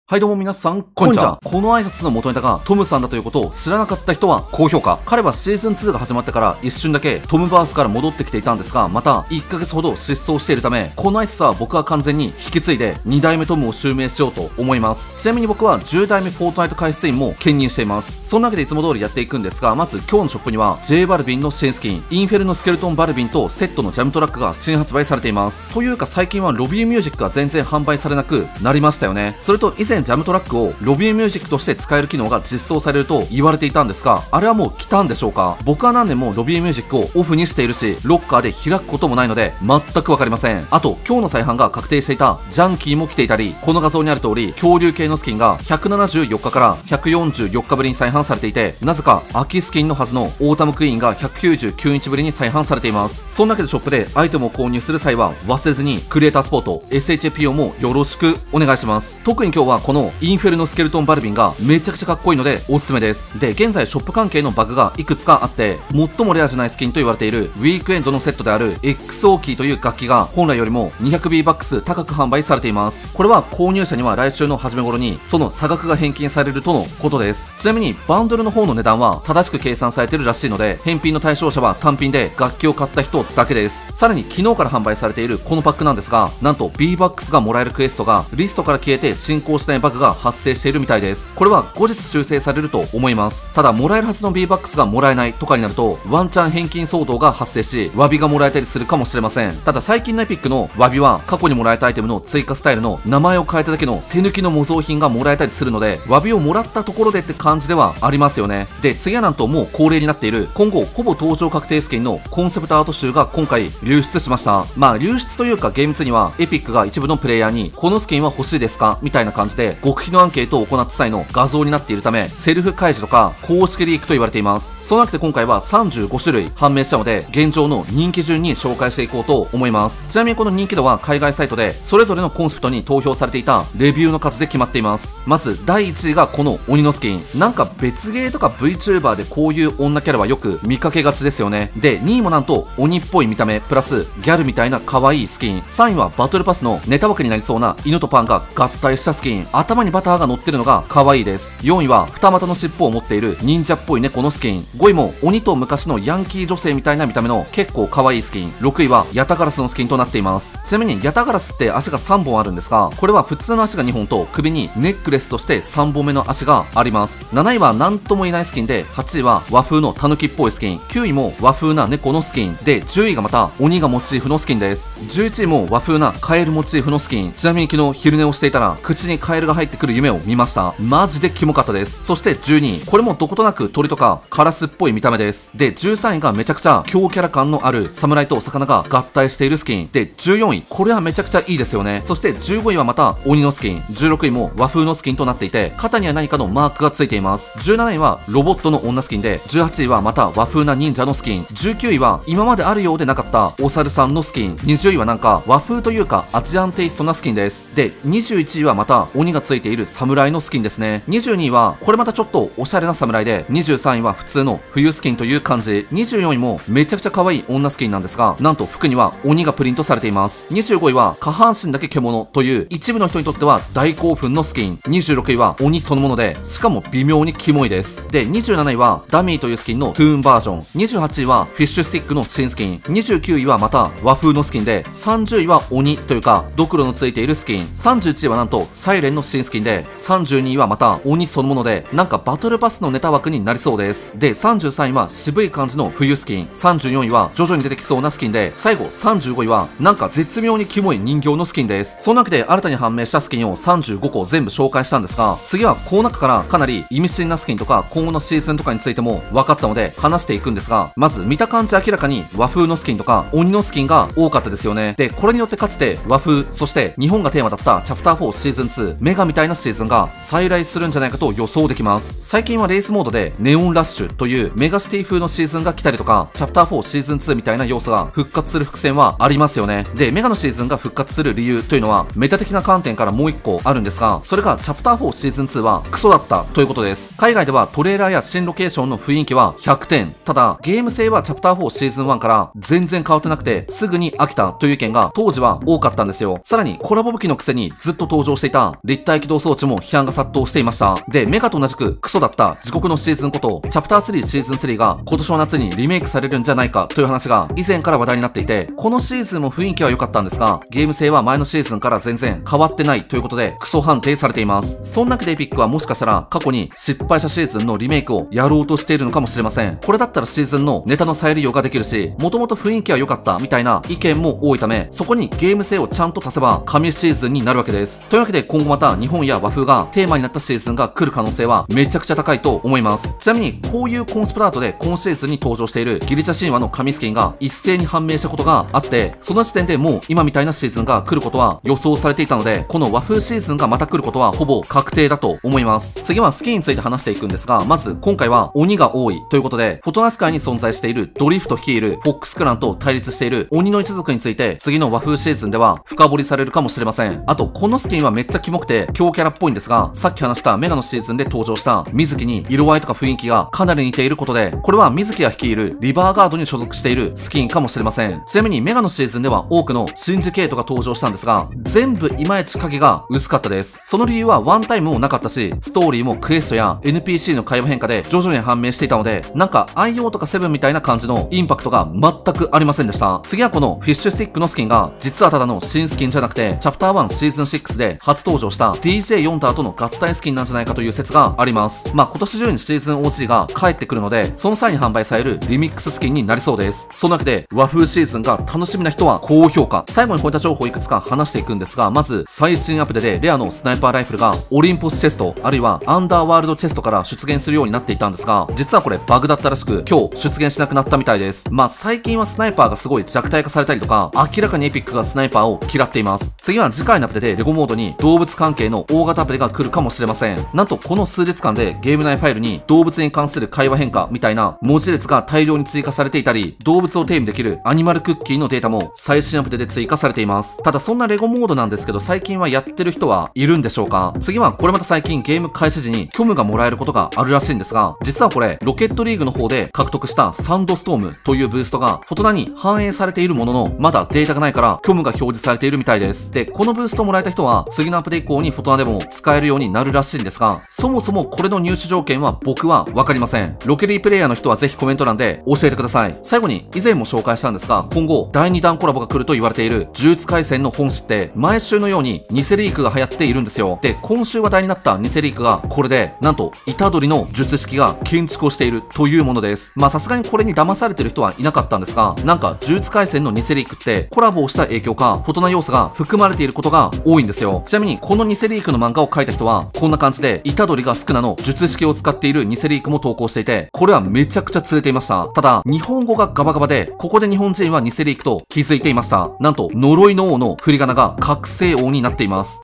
登場確定スキンが大量流出！今後のシーズンや謎報酬も判明！メガシティが復活したり呪術廻戦の漫画でフォトナの建築が描かれた？件も解説！【フォートナイト】【リーク】【無料アイテム】【無料報酬】【新スキン】
クリエイターサポート:SHAPO

BGM使用サイトや使用BGM作曲者様（使用してない場合もあります）